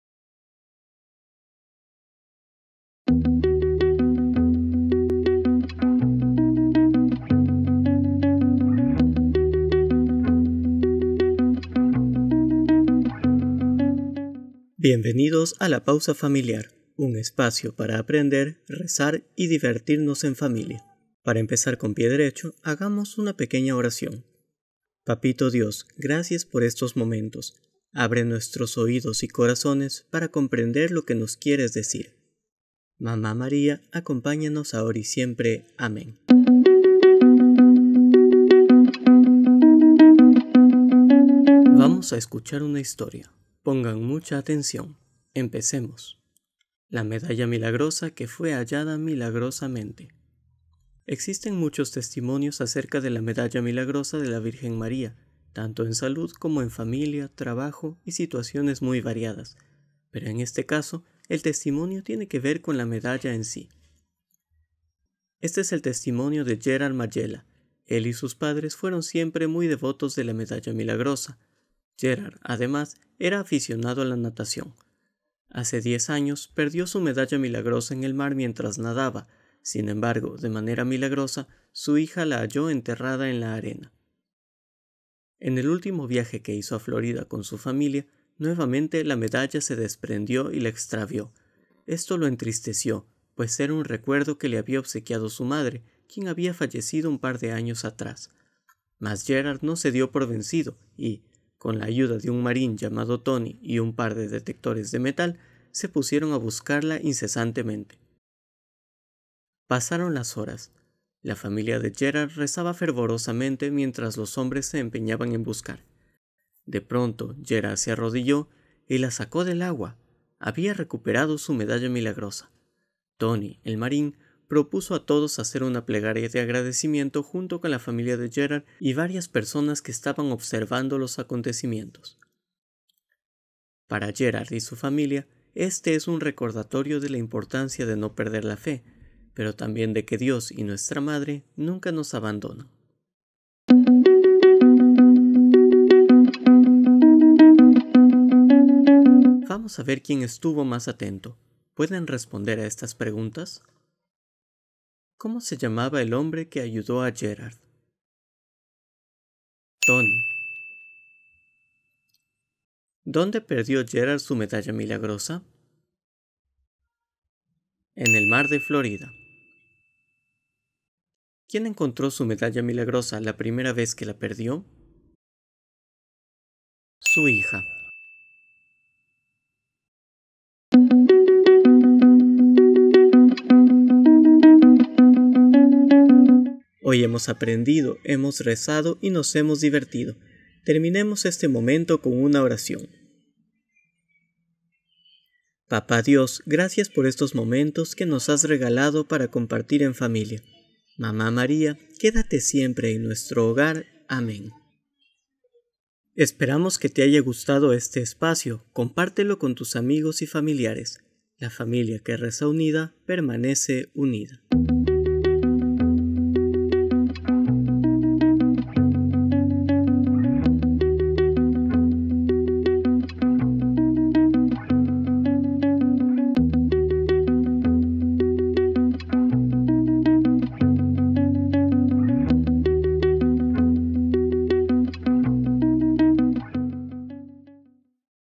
LA PAUSA FAMILIAR episodio 26 - aprendemos, rezamos y nos divertimos en familia
¡La medalla milagrosa 🏅 que fue hallada milagrosamente dos veces! 😱